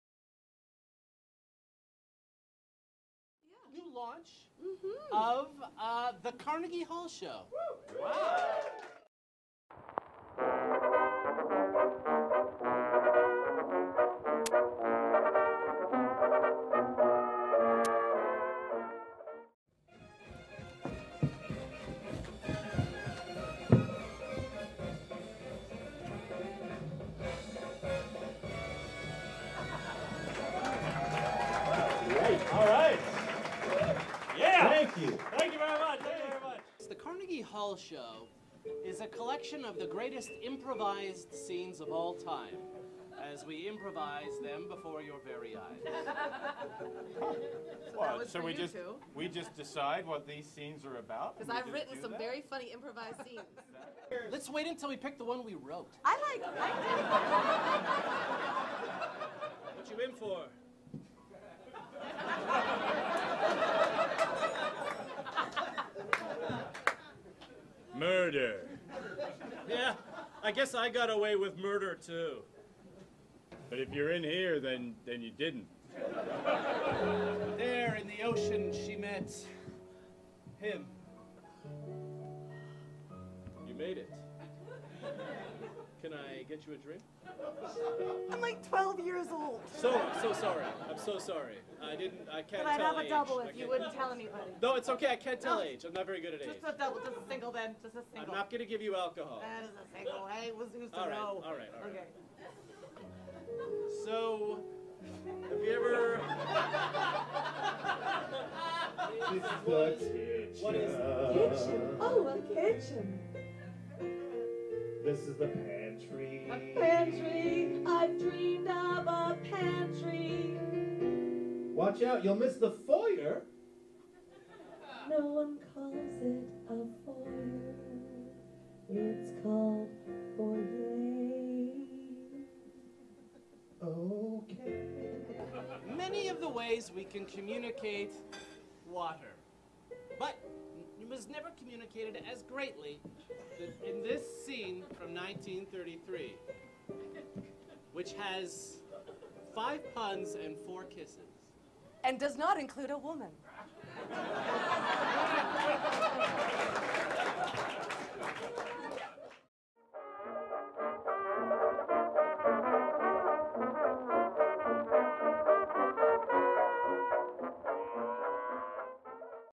The Carnegie Hall Show April 30th, 2014
The Carnegie Hall Show is an award winning improvised retrospective of the greatest improvised scenes of all times!  On the last Wednesday of every month, travel back in time as Second City alumni Matt Baram (CityTV's Seed) and Naomi Snieckus (CBC's Mr. D.) as well as Chris Gibbs (Winnipeg Comedy Festival) look back through the ages to bring you "The Greatest Improvised Scenes Ever Performed"! Made up from scratch before your very eyes-replete with witty dialogue, commercial breaks and occasional emotional breakdowns!

This months special guests are Michael Grajewski, Brandon and Kurt Firla.

The last Wednesday of every month at The Drake Hotel

filmed by Jesse Bostick
edited by Kayleigh Robertson